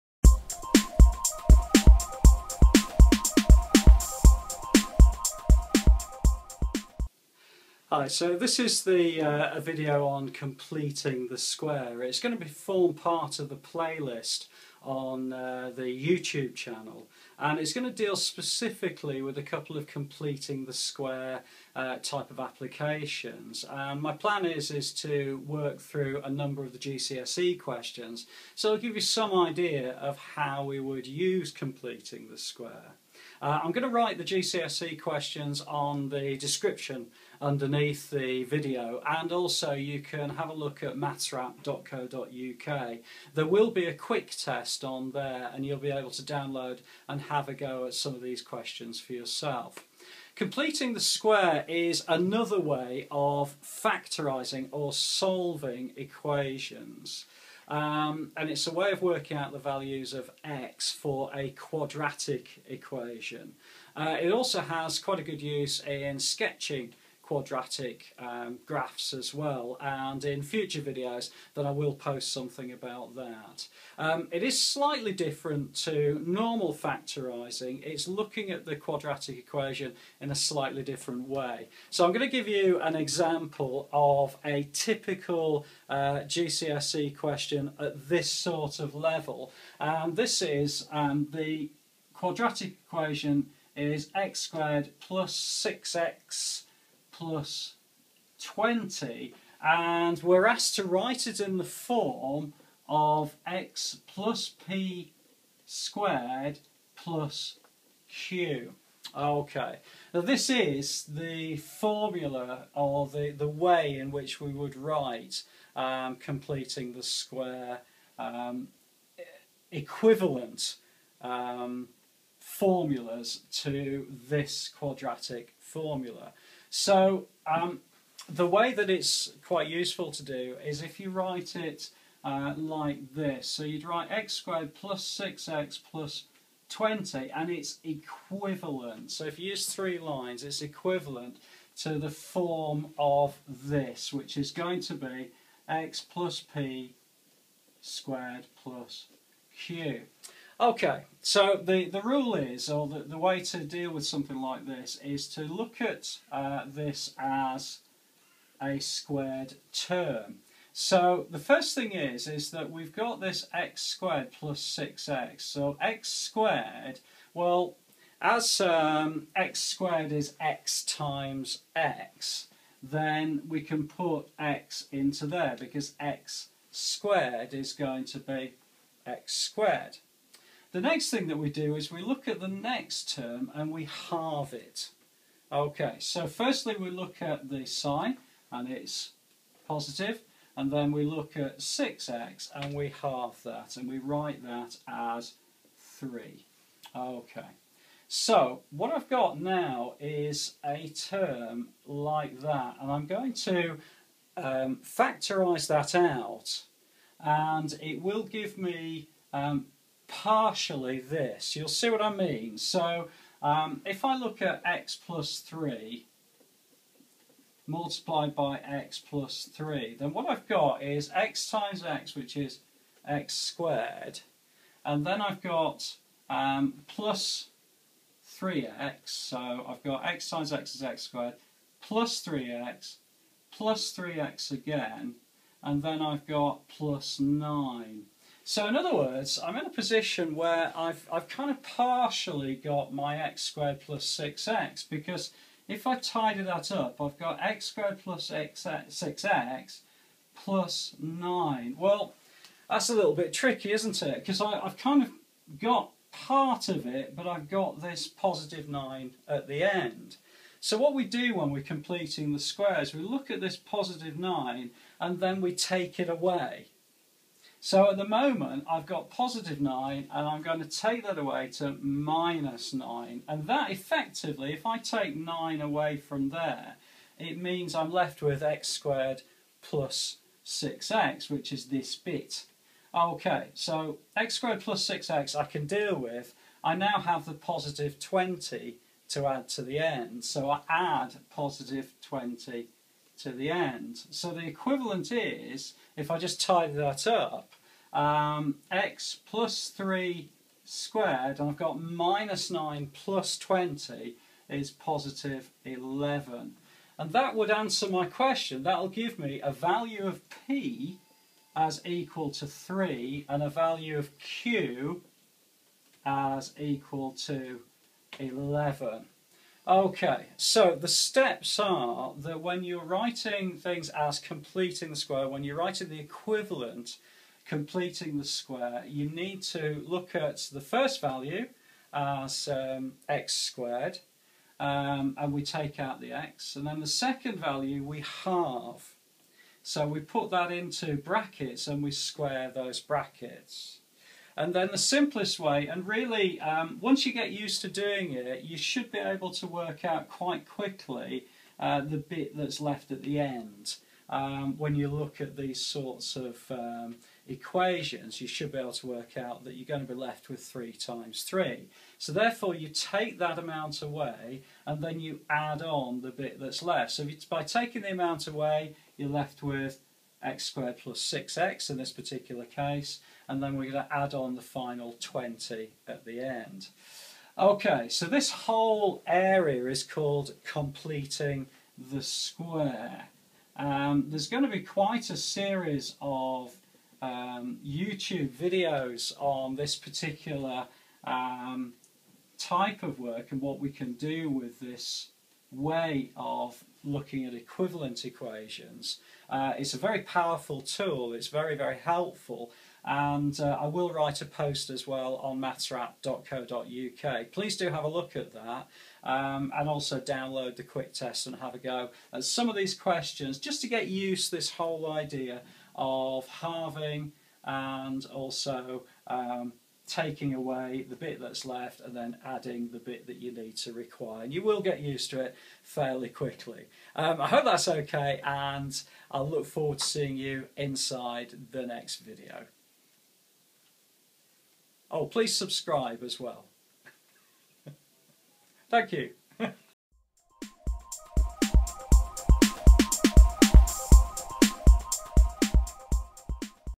Completing the square - introduction
This video introduces 'completing the square' - used in solving and sketching quadratic equations. I've used an exam question from Edexcel grade A GCSE:
"Write x squared + 6x + 20 in the form (x + p)squared + q."

real maths, tips and techniques. The quick tests, mentioned in the video, are